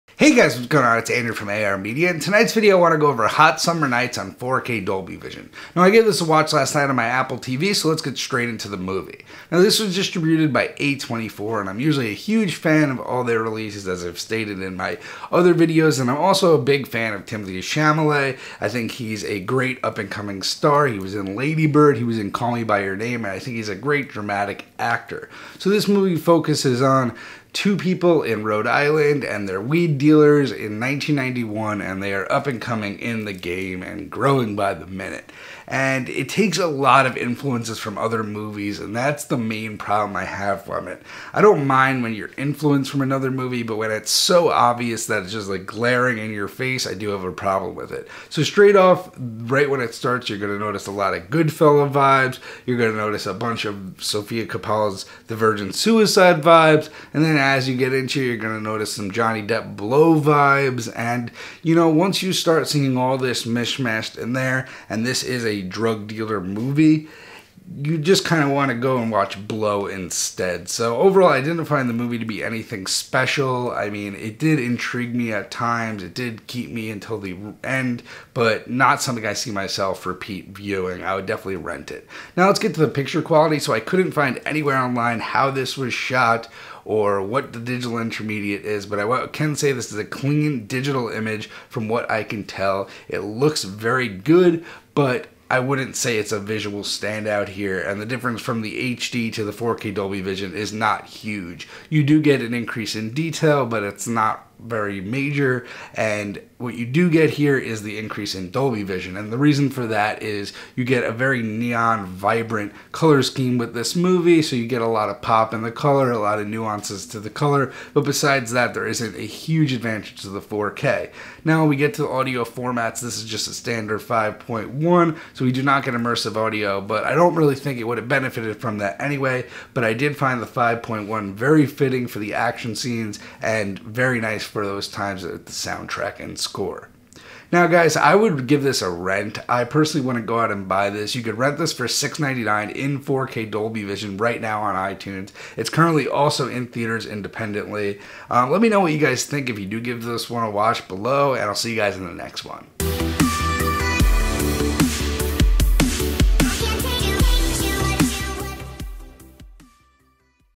Hot Summer Nights 4K Dolby Vision Review
In tonights video I review the new A24 movie Hot Summer Nights on 4K Dolby Vision. In this video I go over the movie itself, the picture quality, the audio quality, and if it's worth the day one pick up!